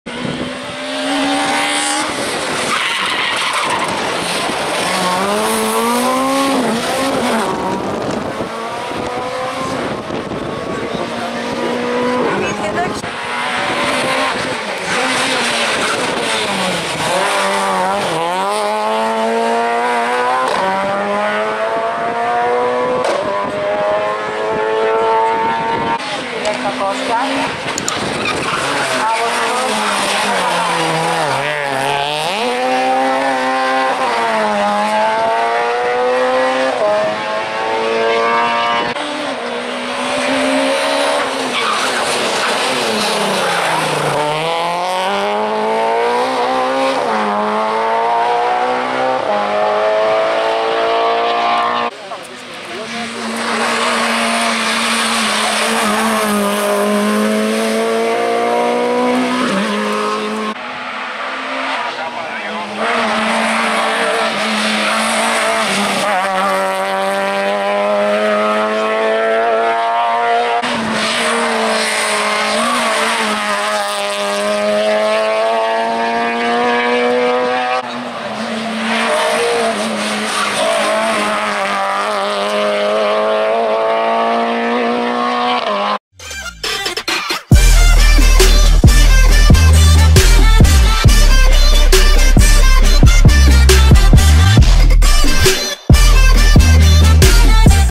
Κλάση Α6 Ανάβαση Πιτίτσας 2019 Πλατάνι – Πιτίτσα Pititsa Hill Climb | MarketNetgr
Κλάση Α6 (Photo: Automotopatras.gr)
Νο 19 ΑΣΜΑ ΜΥΣΙΡΛΑΚΗΣ ΓΕΩΡΓΙΟΣ PEUGEOT 106 S 16 A6
Νο 20 ΑΟΠΑΤΡΑΣ ΑΥΓΕΡΟΠΟΥΛΟΣ ΧΡΗΣΤΟΣ PEUGEOT 106 S 16 A6
Νο 21 ΑΛΑΚΟΡΙΝΘΙΑΣ ΡΕΚΚΑΣ ΒΑΣΙΛΕΙΟΣ CITROEN SAXO VTS A6
Νο 22 ΑΣΜΑ ΜΠΑΤΖΑΝΟΣ ΒΑΣΙΛΕΙΟΣ CITROEN SAXO VTS A6

ΑΝΑΒΑΣΗ ΠΙΤΙΤΣΑΣ ΠΑΝΕΛΛΗΝΙΟ ΠΡΩΤΑΘΛΗΜΑ ΑΝΑΒΑΣΕΩΝ & ΙΣΤΟΡΙΚΩΝ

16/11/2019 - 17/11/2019
8ος γύρος Πανελληνίου Πρωταθλήματος Αναβάσεων
Διοργάνωση: ΑΟΠ